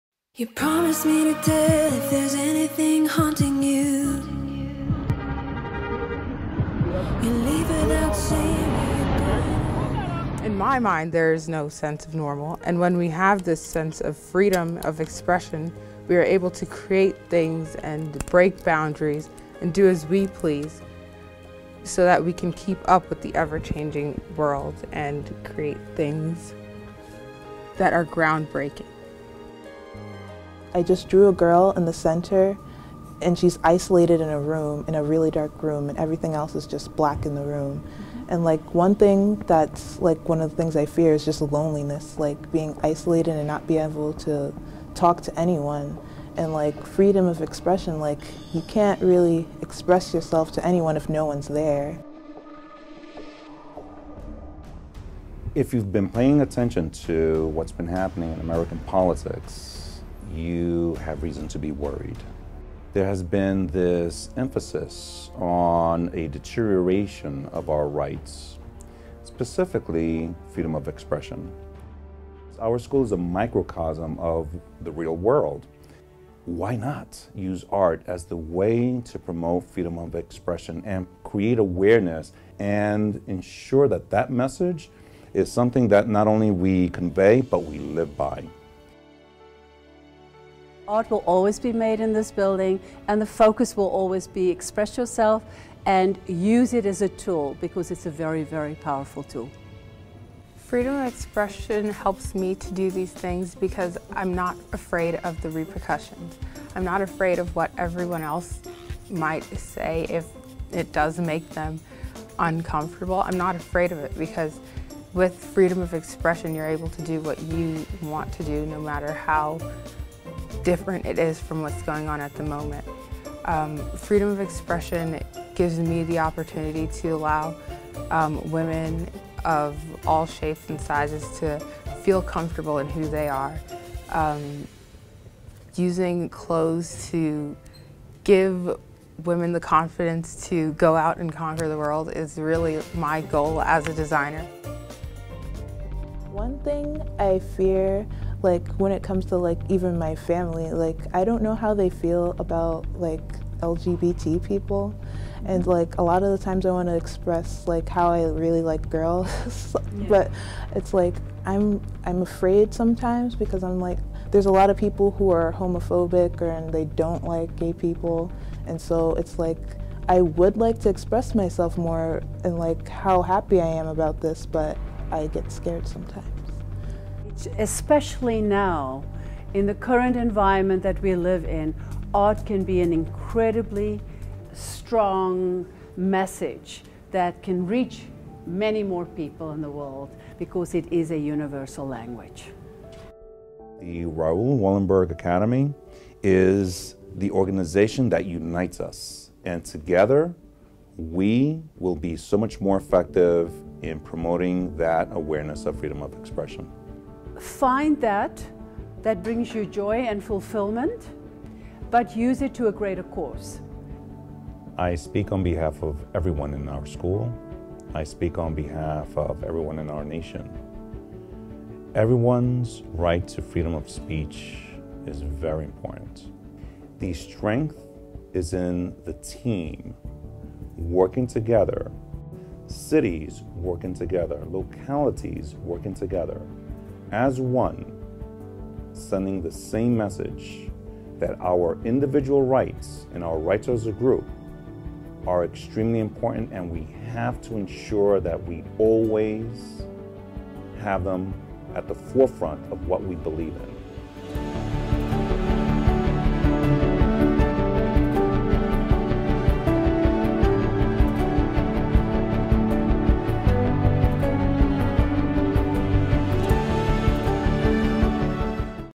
The Cube Method in New York
High School of Art & Design in New York takes part of the the Cube Method, a project that aims to inform students about human rights and equality. A black “cube” (2*2*2 meters) is sent to selected high schools around Sweden, and for the first time in 2017: also schools in New York, Paris, Belgrade and Budapest. This is made possible trough a cooperation with the Swedish Institute. The film is made by Carl Eneroth, Stockholm Social Innovation Lab.